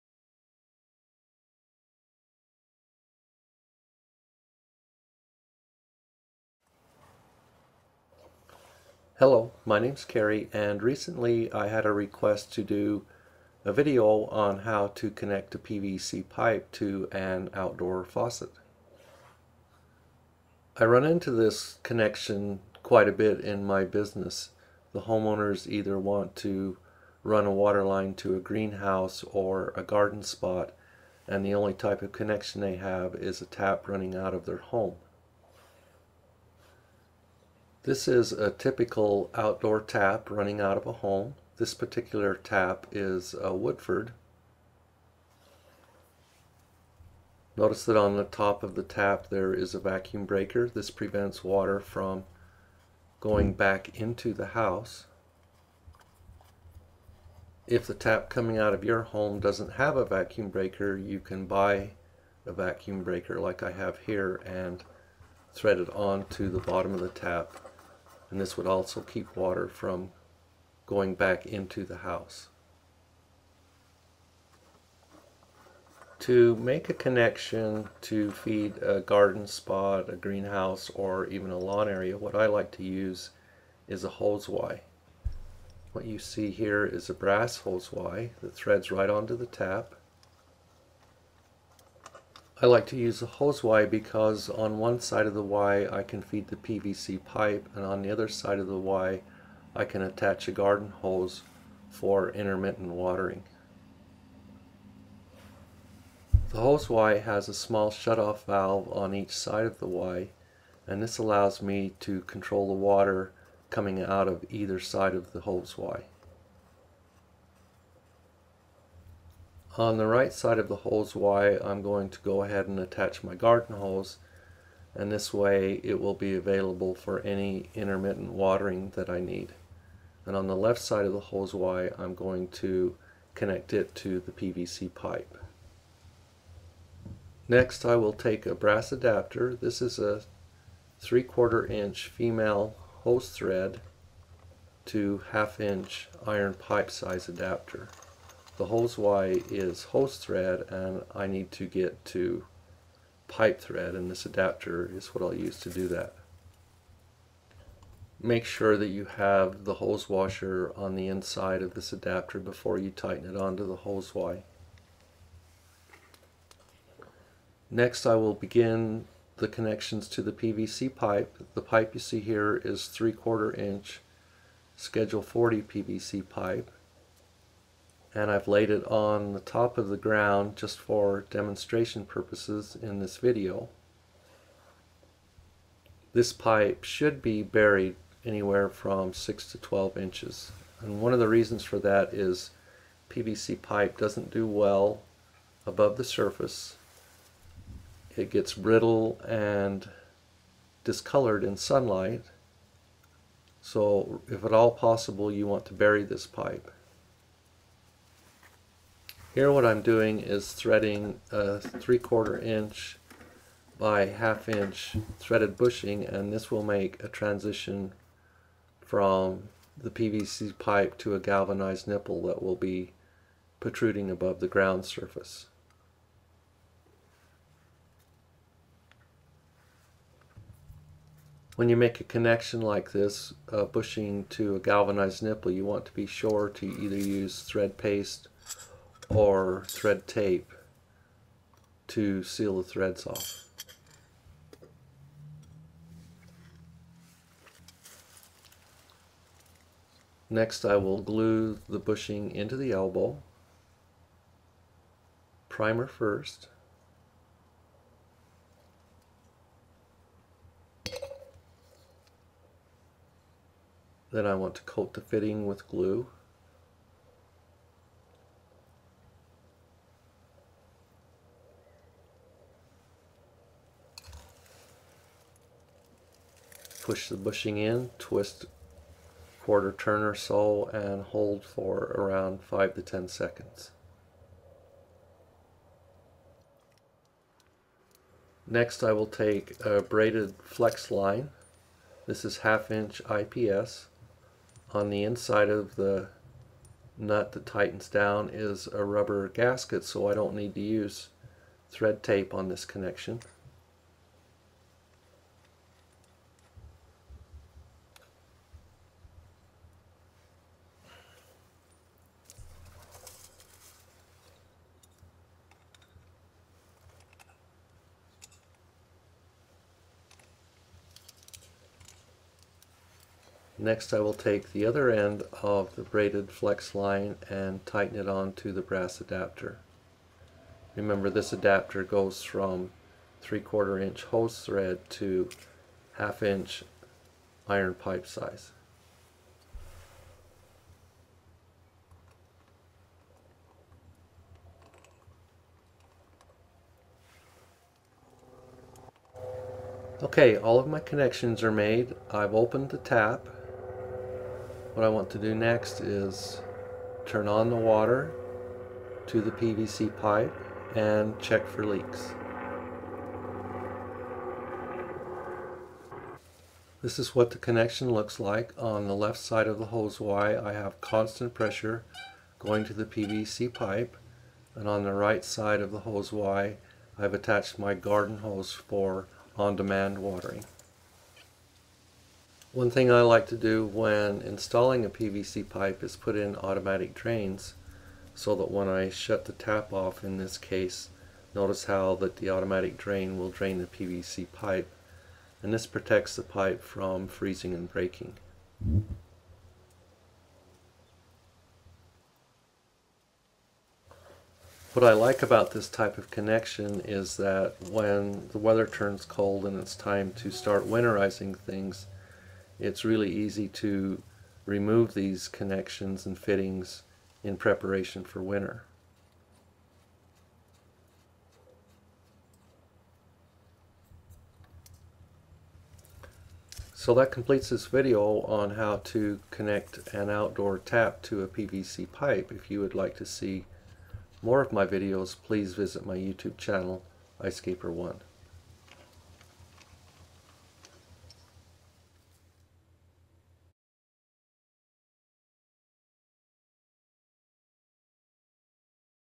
How To Connect A PVC Pipe To An Outdoor Tap/Faucet
Step by step instructions for connecting a PVC pipe to an outdoor tap.

I see this type of connection a lot in my business. Homeowners want to connect a line with constant pressure to a greenhouse, garden spot, or lawn area. Video covers how to make this type of connection.

Links to items mentioned in the video:

3/4” Brass Hose Wye
3/4” FHT (Female Hose Thread) x 1/2” MIPT (Male Iron Pipe Thread) Brass Adapter
3/4” x 1/2” PVC Threaded Bushing SxT (Slip x Thread)
1/2” x 12” Galvanized Nipple
1/2” Braided IPS Braided Flex Line
3/4” x 3/4” x 1/2” PVC SCH40 Tee SxSxT

On a connection of this type make sure the tap has some type of backflow prevention to keep water from being able to enter the home.